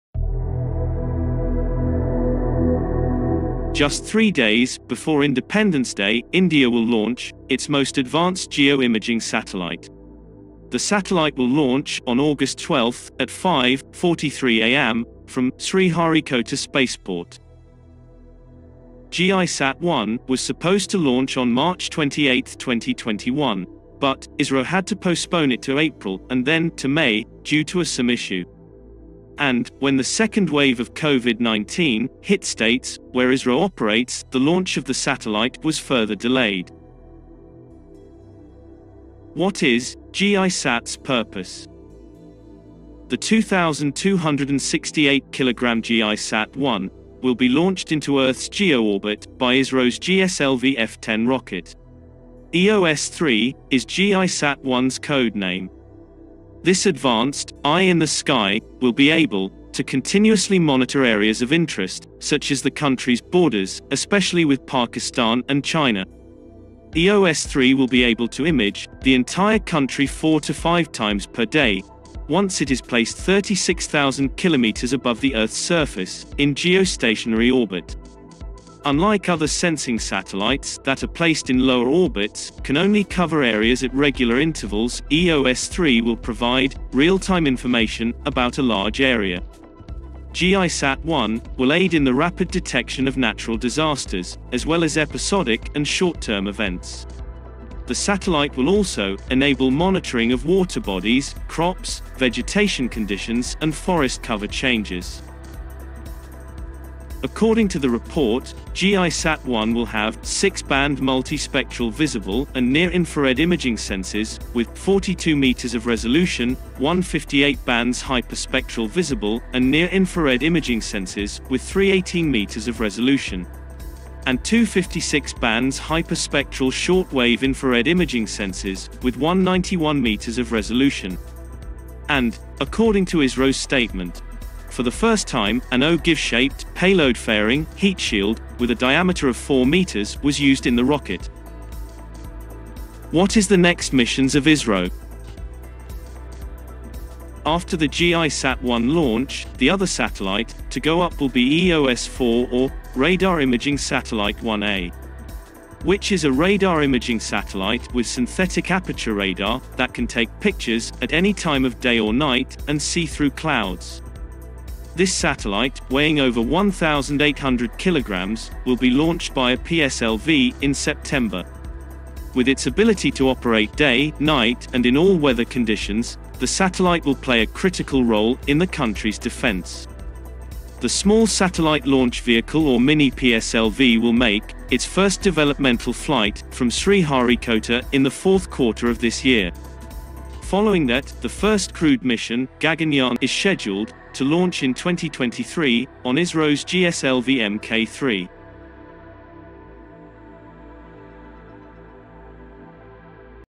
India To Launch Advanced Geo Imaging Satellite (EYE IN THE SKY)
The advanced ‘eye in the sky’ can monitor areas of interest and give real time information about a large area unlike other remote sensing satellites placed in lower orbits that come to a spot only at regular intervals, TOI noted.

As per media reports, a 4-metre diameter ogive-shaped payload fairing (heat shield) has been used in the rocket for the first time. In addition, the satellite is set to have payload imaging sensors of six-band multispectral visible and near infra-red with 42-metre resolution.

Following the launch of Gisat-1, India will be focussing on the launch of EOS-4 or Risat-1A, which is a radar imaging satellite and can take pictures day and night and can also see through clouds by a PSLV in September. This satellite is set to play a strategic role in the country’s defense by virtue of its capability to operate throughout the day and in all weather conditions alike.